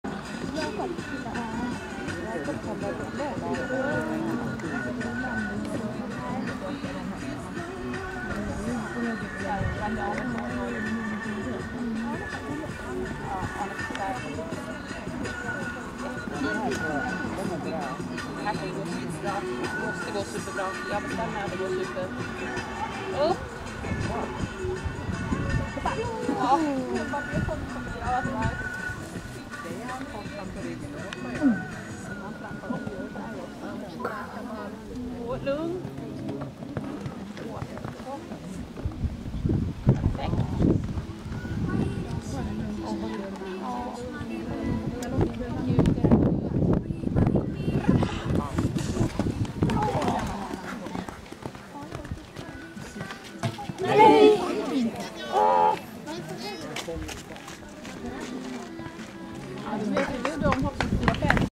KILLEA FEAR BUI
Jacob och Arne rider final i Norrlandsmästerskapet 2016